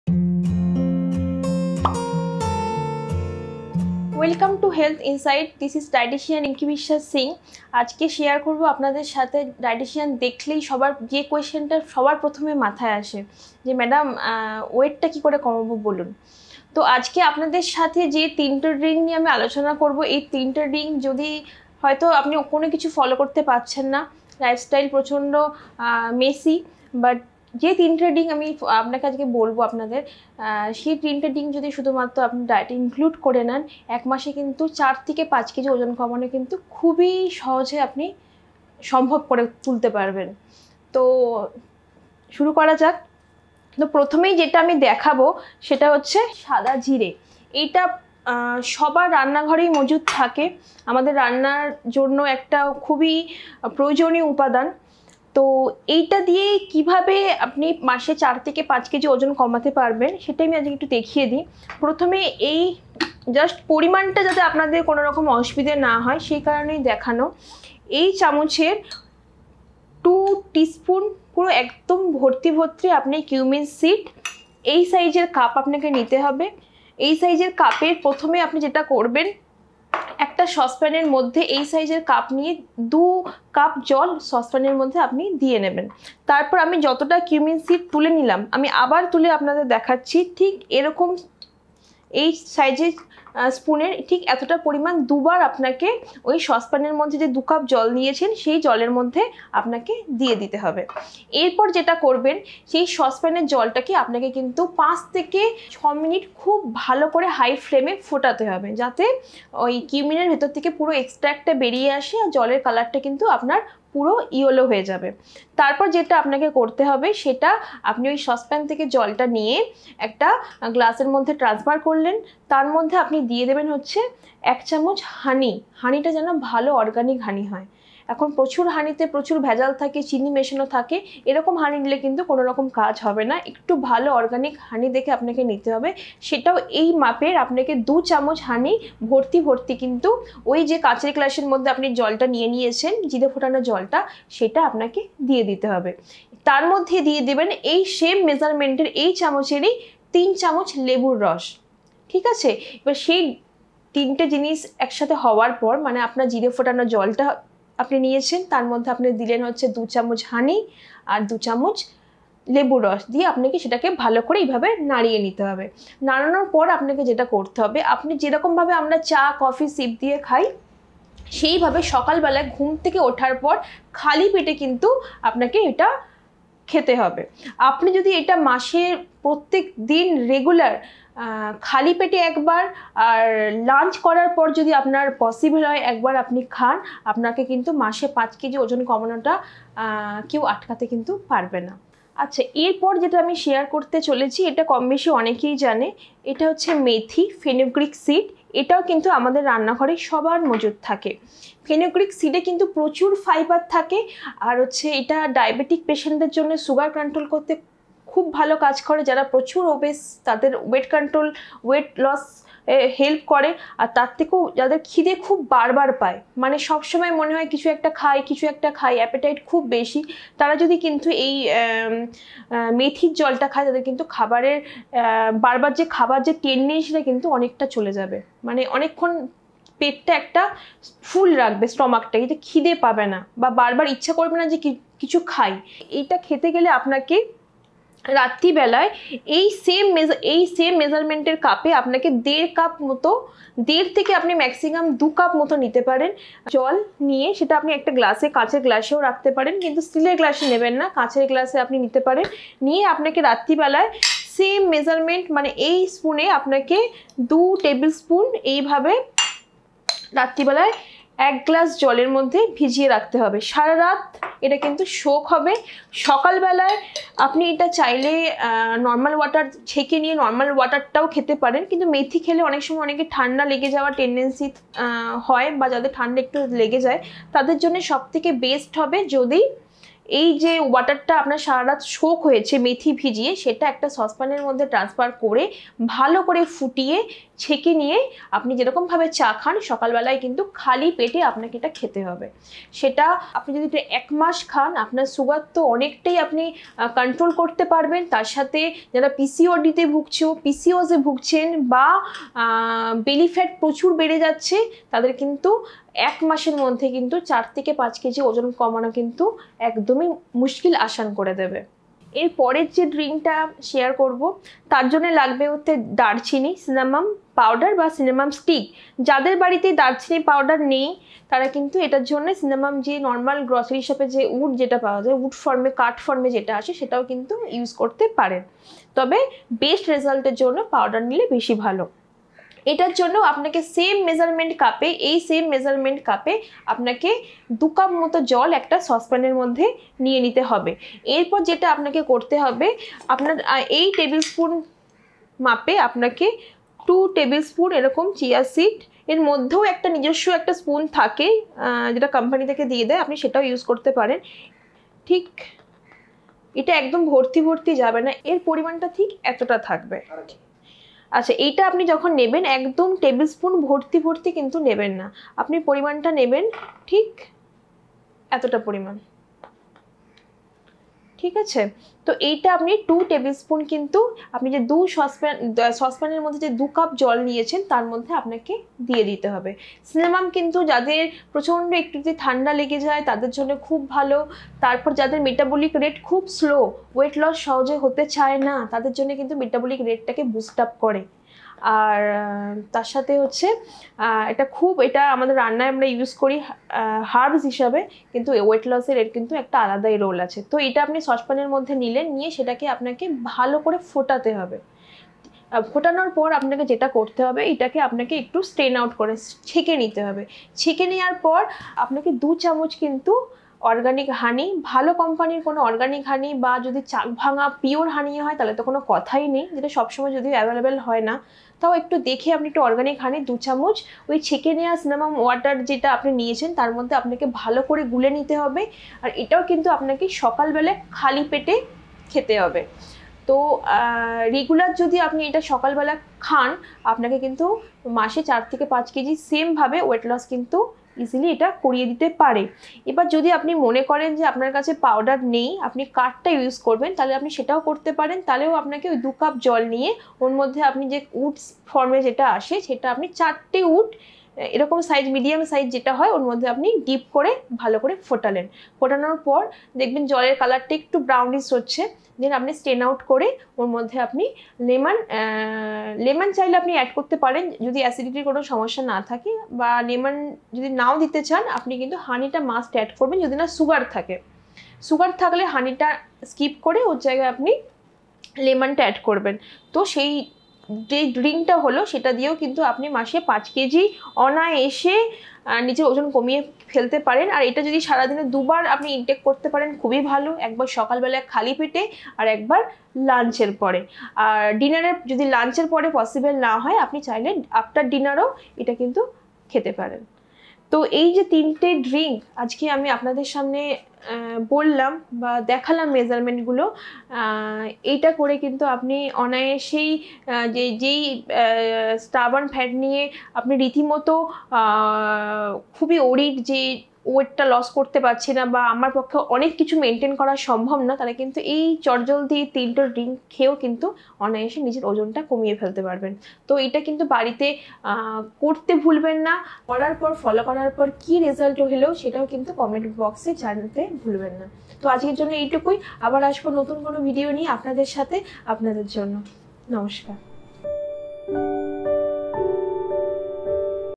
অতিরিক্ত মেদ ও ওজন কমানোর সহজ উপায় | How to lose weight fast? weight loss tips in Bengali
অতিরিক্ত মেদ ও ওজন কমানোর সহজ উপায় | How to lose weight fast? weight loss tips in Bengali by dietician Rinki Biswas Singh
ডায়েট সংক্রান্ত যেকোনো বিষয় যোগাযোগ করুন:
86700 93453 (For Appointment Only)